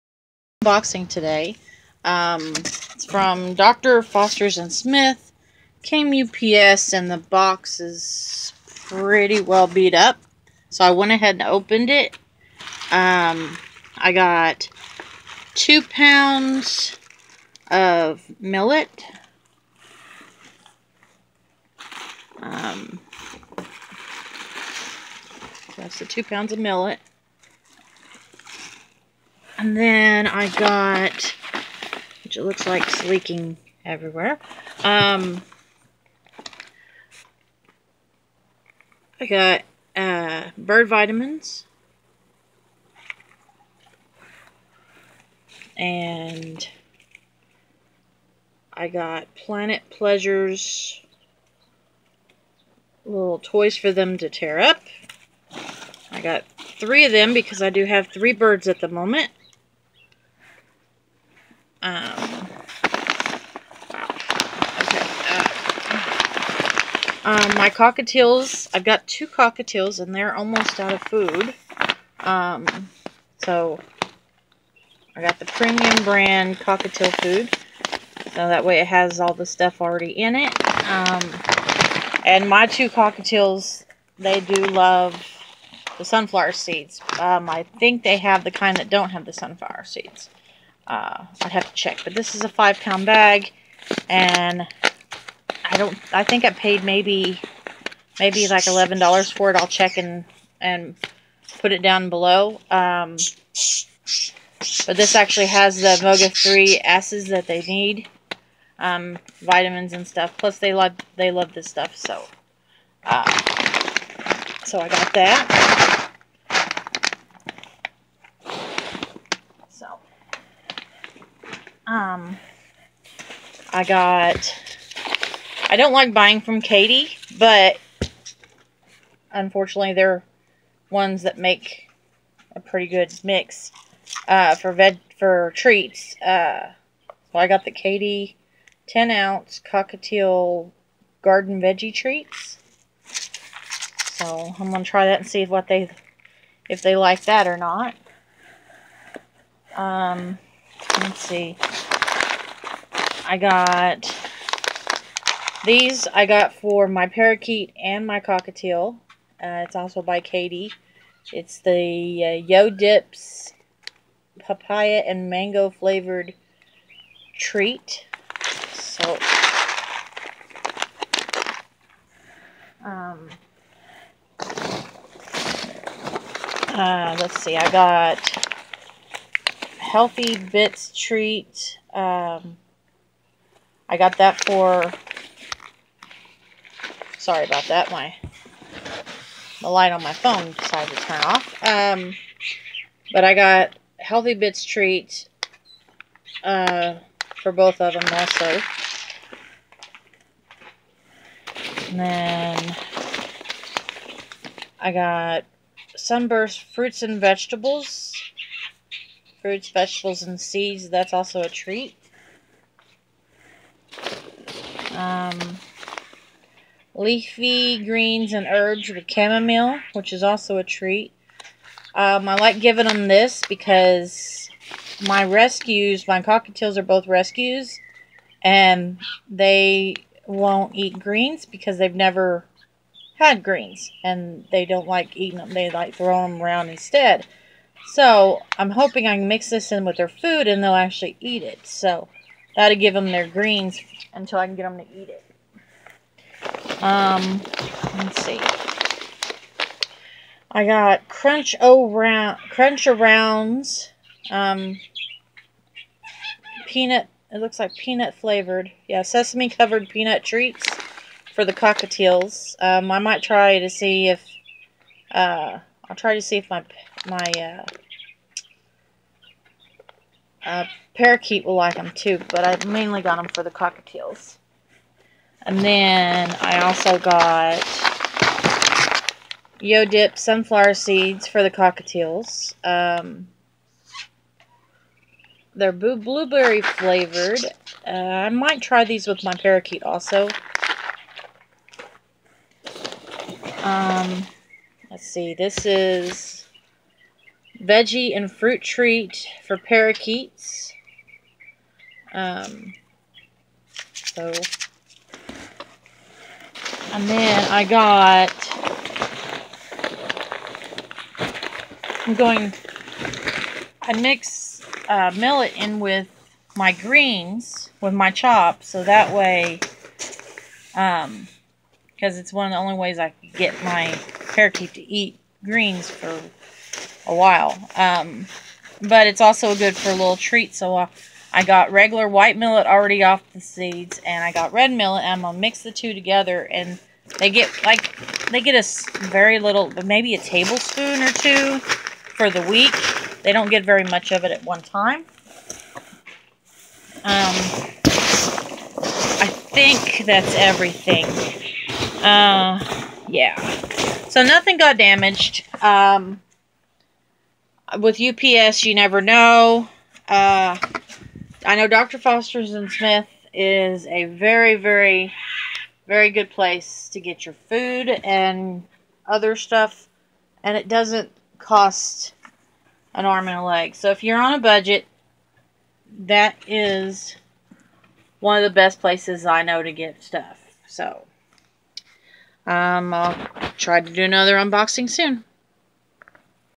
Bird supplies unboxing from Dr Foster's and Smith
Dr Foster's and Smith unboxing of bird supplies for my parakeet and 2 cockatiels.